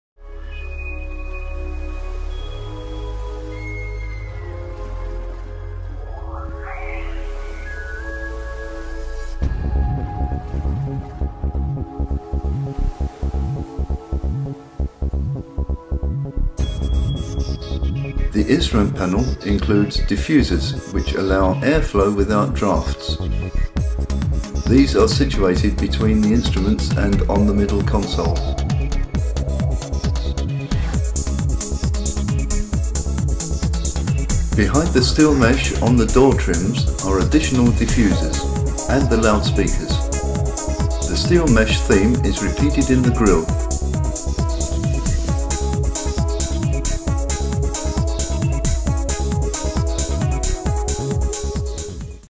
CONCEPT CAR AIR VENT DIFFUSORS
The Bayliss Graphics Concept Car was designed to show the possibilities available to our customers, without showing secret designs specific to any one customer.
Our customers at this time were nearly the complete supplier industry in Germany and several OEMs.
This was first shown at the Euromold exhibition in Frankfurt in 2001.